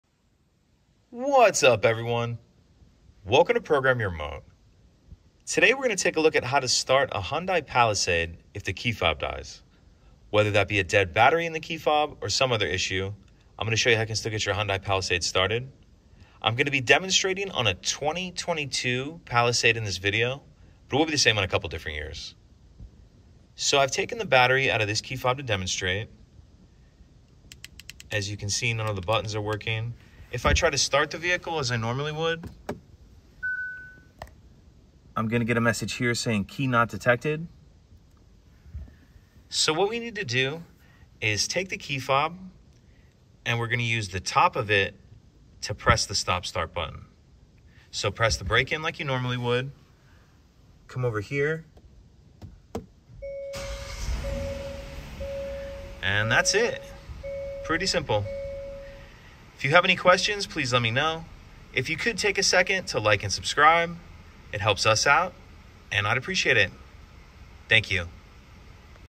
2020 - 2022 Hyundai Palisade Key Not Detected - How To Start With Dead, Bad Key Fob Battery
A video tutorial on how to start a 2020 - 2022 Hyundai Palisade with a remote key fob that has a dead battery or is showing the error message Key Not Detected.

Hyundai gives the following information on what to do if your Hyundai Palisade has a dead or broken remote key fob and won't start - showing the message Key Not Detected :

In case the ignition switch does not change with the push of a button, the key fob may have a dead
or depleted battery. In this situation, a back up method can be used to operate the ignition switch. Put the Top of the key fob against the Hyundai Palisade START/STOP ignition button and push to operate the ignition key switch.  The key fob may not be able to be detected by the Hyundai Palisade key fob system if it is located next to a mobile phone, laptop or other electronic device; these devices may prevent the key fob’s wireless signal from being detected and prevent the system from starting your Hyundai Palisade.

This video tutorial will show you how to start your vehicle with the error message showing "Key Not Detected" on the following Hyundai Models:
2020 Hyundai Palisade
2021 Hyundai Palisade
2022 Hyundai Palisade